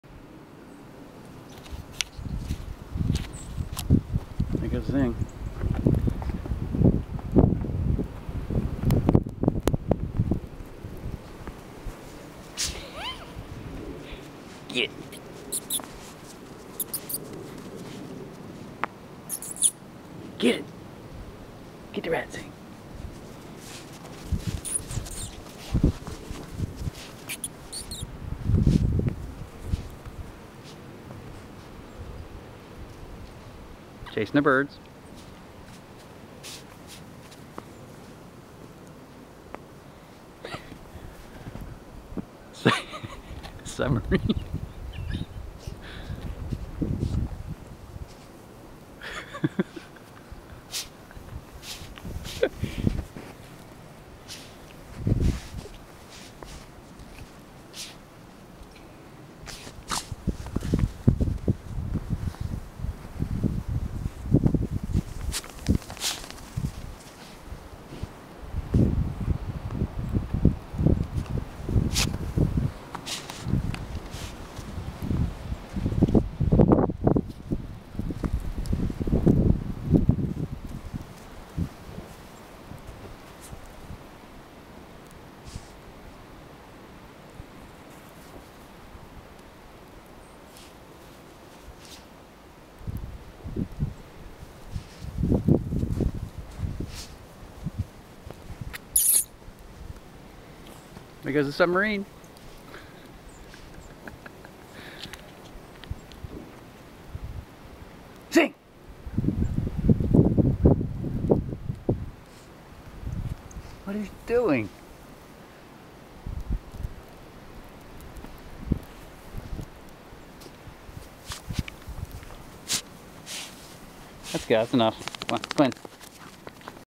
Jack Russell terrier go to ground in deep snow
"Raw" video of our JRT's playing in the 20"+ snowfall from the 6 FEB Mid-Atlantic snow storm.  Our male likes to "go to ground" in it and looks like a submarine plowing thru the snow cap.